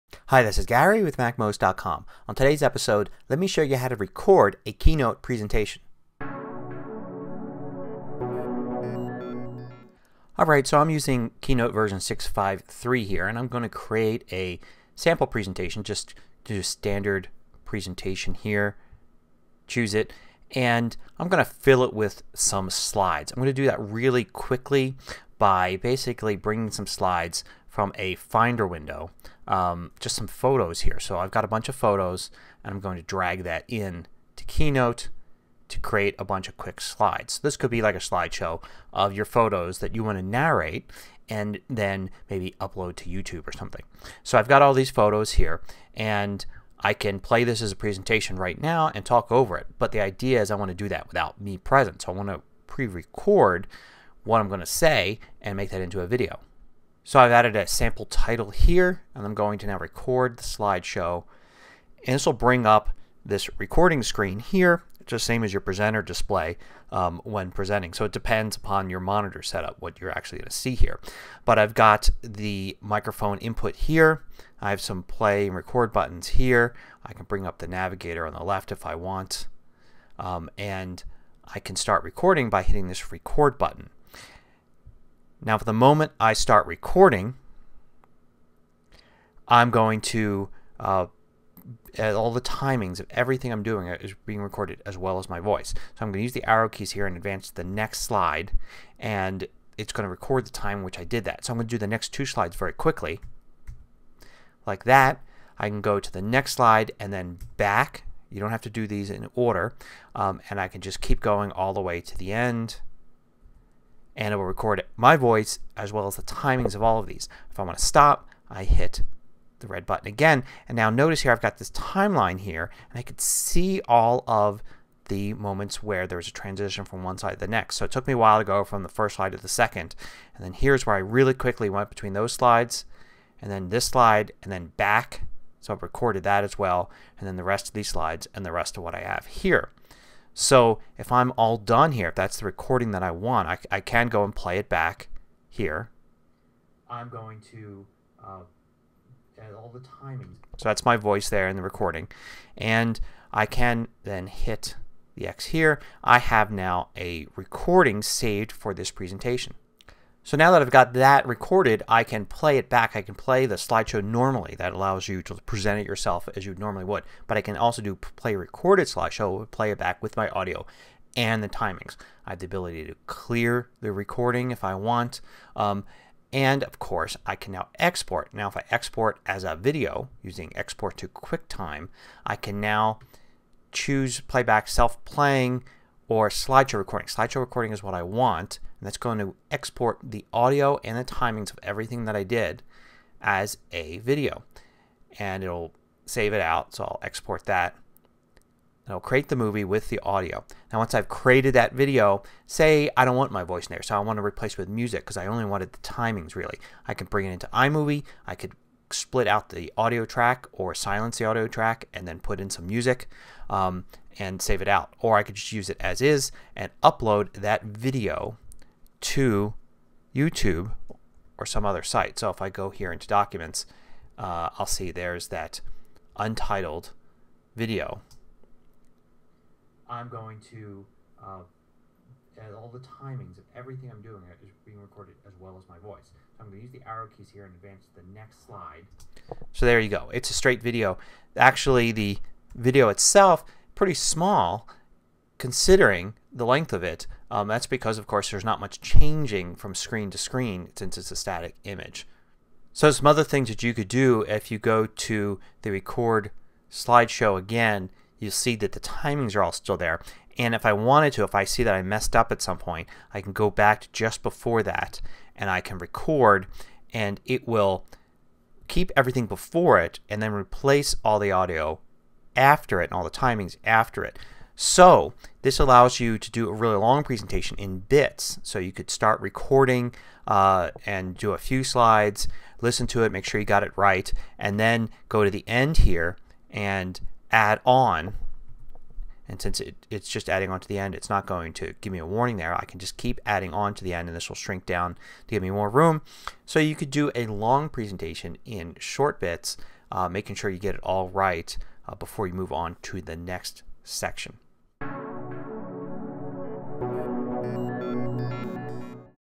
Keynote Voiceover Recording (#1119)
You can record your voice and the timing between slides inside Keynote and play the recording back. You can also export the recording as a video. The recording does not have to show the slides in order. You can also record in sections, moving on to the next section after checking to make sure the previous sections are good. Once you export to video, you can bring the video into iMovie to add music.